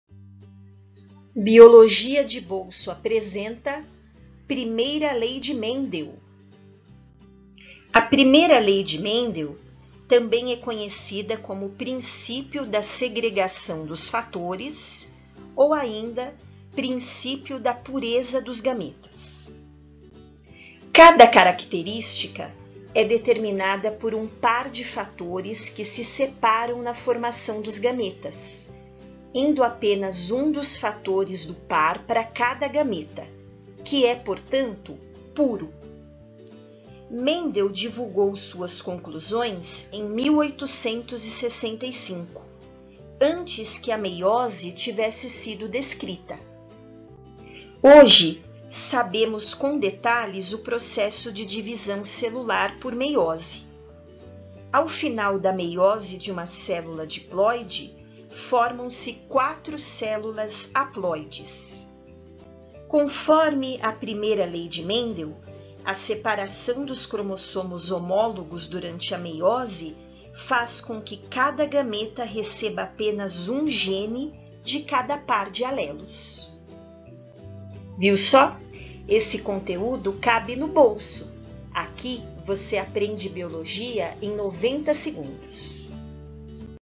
Primeira Lei de Mendel
A Primeira Lei de Mendel é também conhecida como "Princípio da Segregação dos Fatores". Assista e entenda!!

São conteúdos que cabem no bolso. Aqui, você aprende Biologia em 90 segundos!!